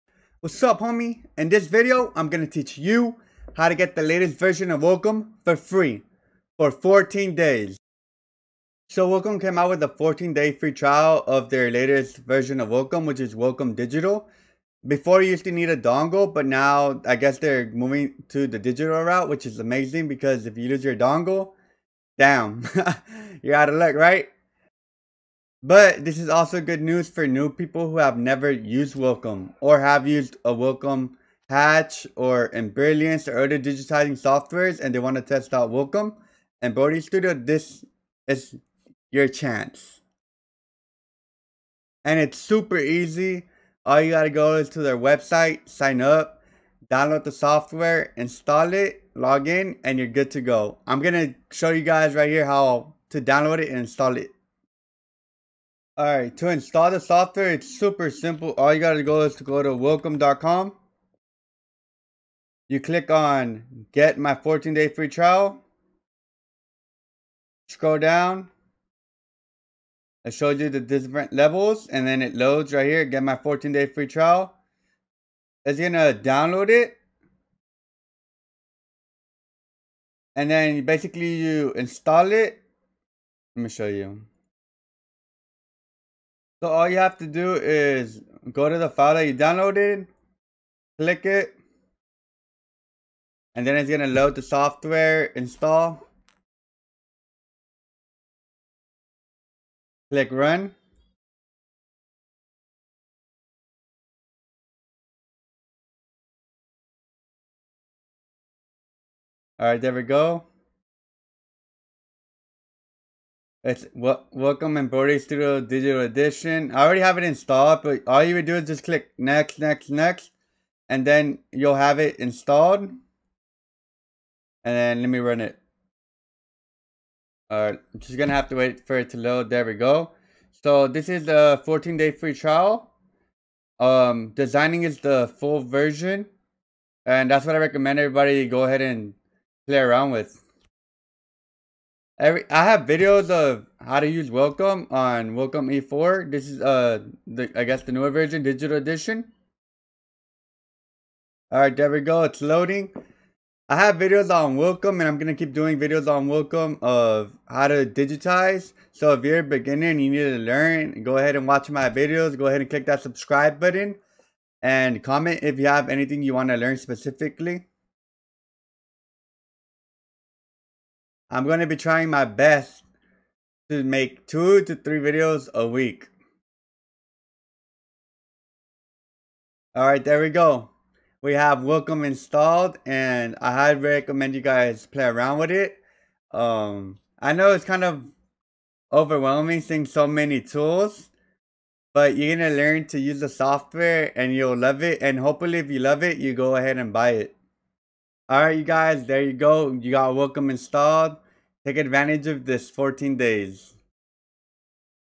How To Install WILCOM ! e4.5 FOR FREE FULL TUTORIAL | STEP BY STEP
In this video i give an update on the available software in the embroidery industry & how to install it.